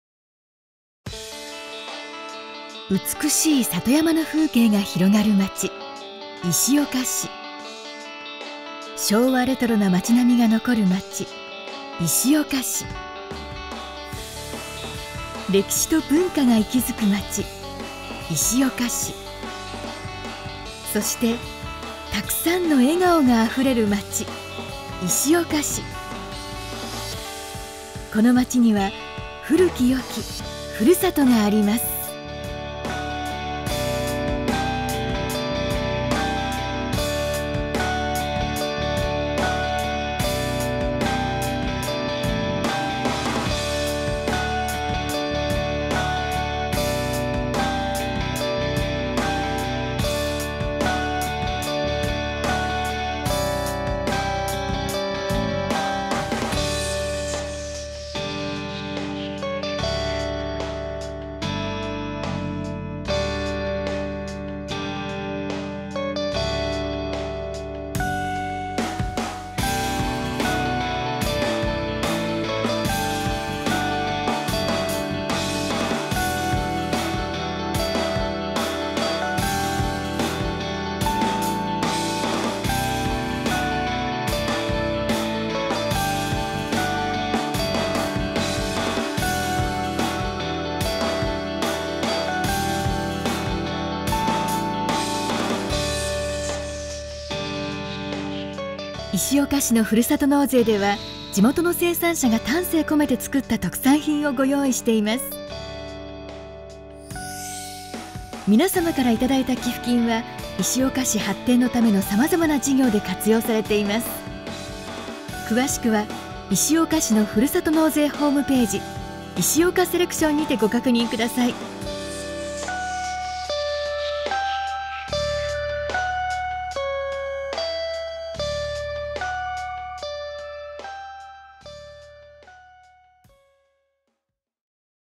【茨城県石岡市公式】 石岡市ふるさと応援寄附
豊かな自然のなかで、元気に遊ぶ子どもたち。自然とともに生きる暮らし。石岡市には、未来に引き継いでいきたい暮らしと風景があります。石岡市が現在実施している、ふるさと応援寄附。応援してくださるみなさんには、御礼として、農産物やお酒、味噌、果物や工芸品など、石岡市の特産品をお贈りしています。

石岡市ふるさと応援寄附／いしおかセレクション